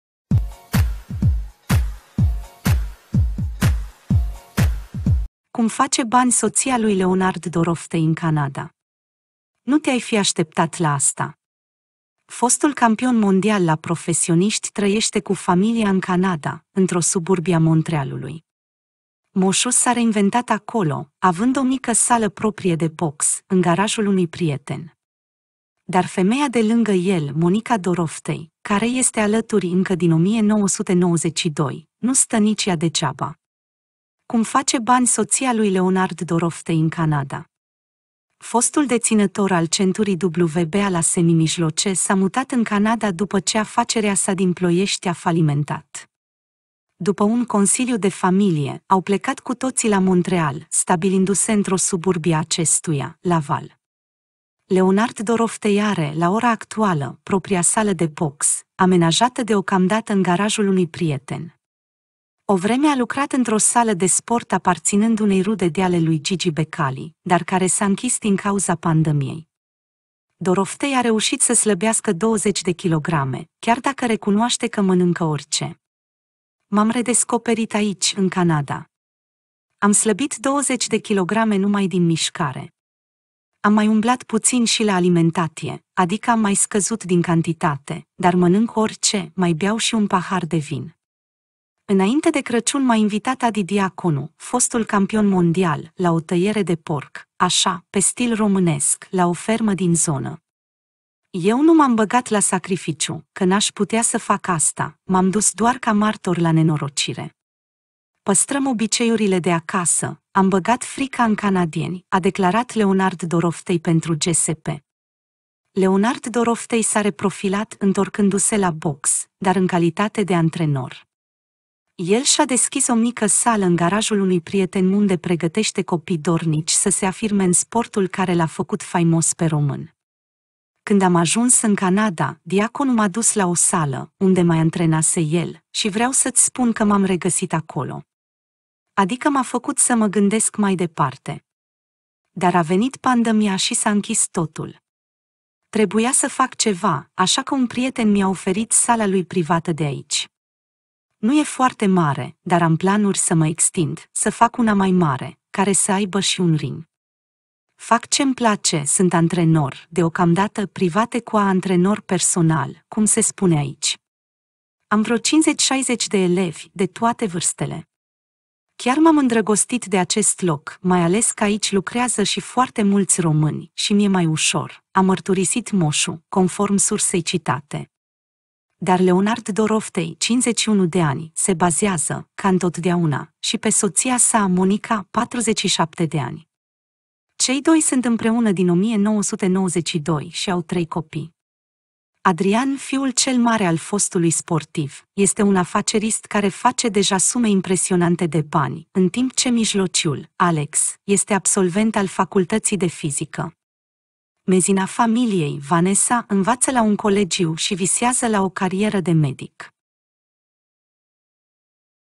Cum face bani soția lui Leonard Doroftei în Canada. Nu te-ai fi așteptat la asta
Fostul campion mondial la profesioniști trăiește cu familia în Canada, într-o suburbie a Montrealului. „Moșu” s-a reinventat acolo, având o mică sală proprie de box, în garajul unui prieten. Dar femeia de lângă el, Monica Doroftei, care-i este alături încă din 1992, nu stă nici ea degeaba.